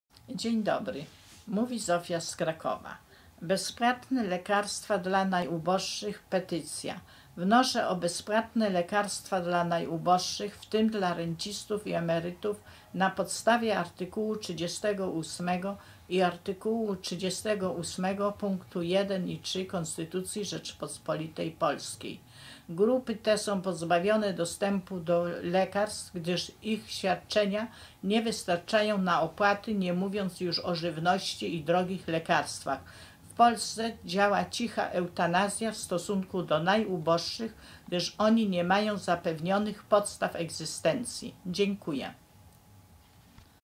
Petycja - bezpłatne lekarstwa dla najuboższych
Wnoszę o bezpłatne lekarstwa dla najuboższych, w tym dla rencistów i emerytów na podstawie artykułu 38 i artykułu 68,1,3 Konstytucji RP. Grupy te są pozbawione dostępu do lekarstw, gdyż ich świadczenia nie wystarczają na opłaty, nie mówiąc już o żywności i drogich lekarstwach. W Polsce działa cicha eutanazja w stosunku do najuboższych, gdyż oni nie mają zapewnionych podstaw egzystencji